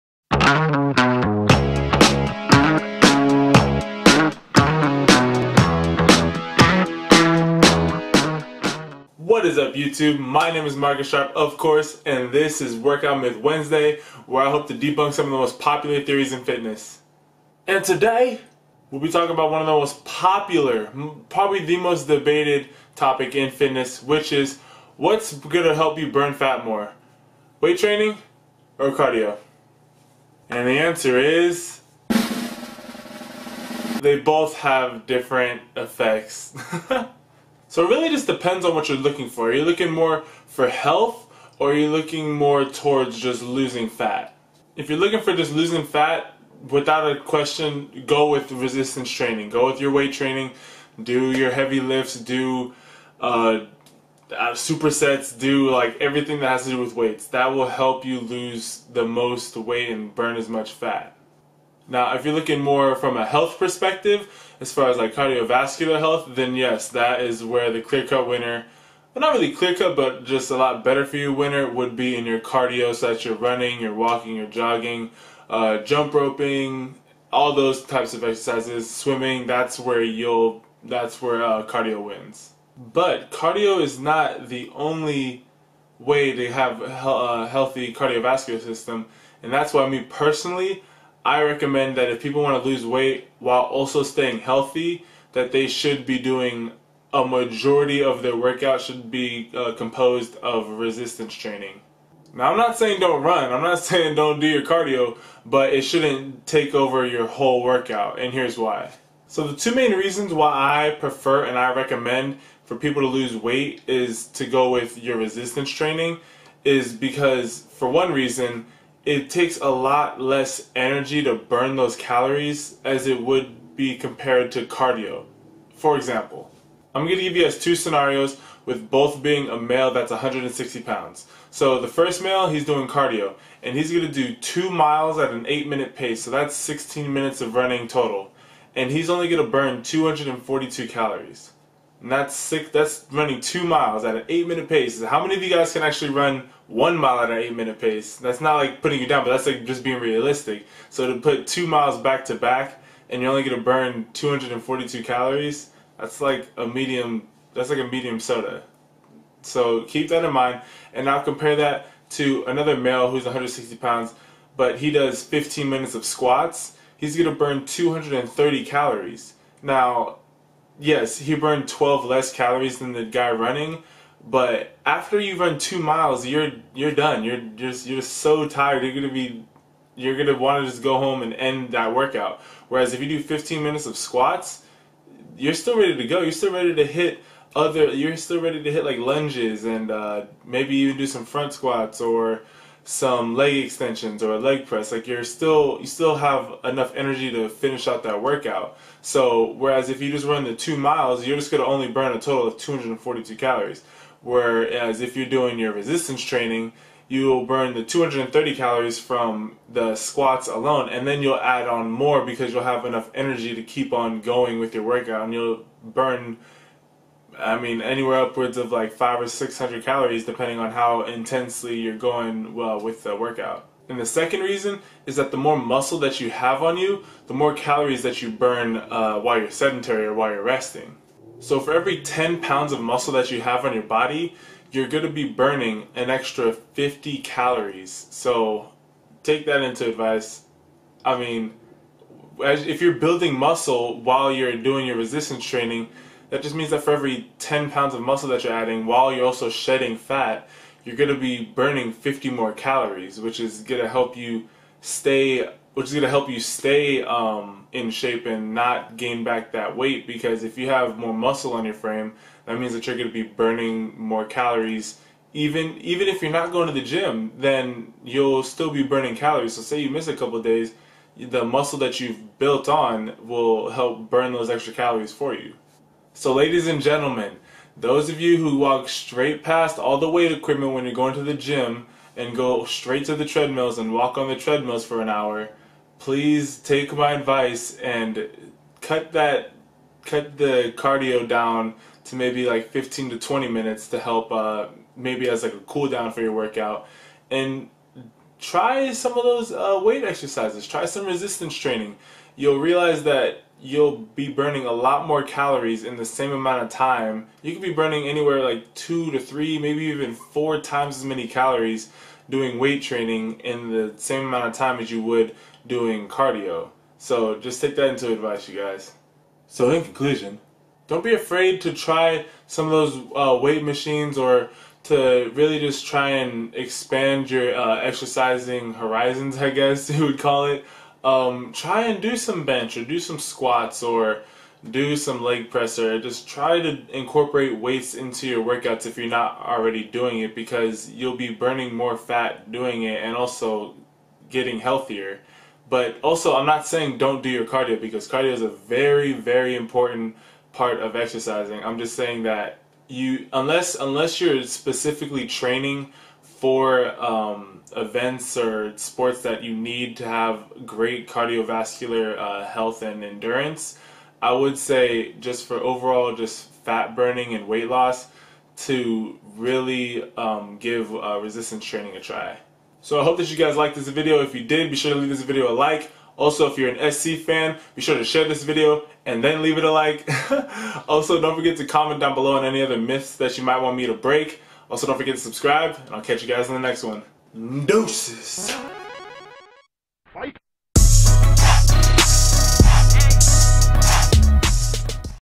STOP DOING CARDIO TO LOSE FAT???: Workout Myth Wednesday | Sharpe Sports
If you haven't already you should be incorporating a lot of resistance training in you workouts to best burn fat. While cardio is good in developing cardiovascular endurance and promotes heart health, resistance training is the best way to lose fat.